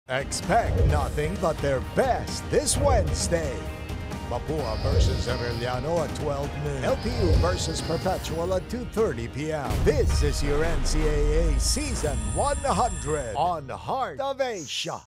Watch these NCAA games today on Heart of Asia
It's game day Wednesday as the Mapua Cardinals face the Arellano Chiefs at 12 p.m., while the LPU Pirates and Perpetual Altas will battle it out at 2:30 p.m. Catch the games live on Heart of Asia, as well as NCAA Philippines, and GMA Sports PH social media platforms.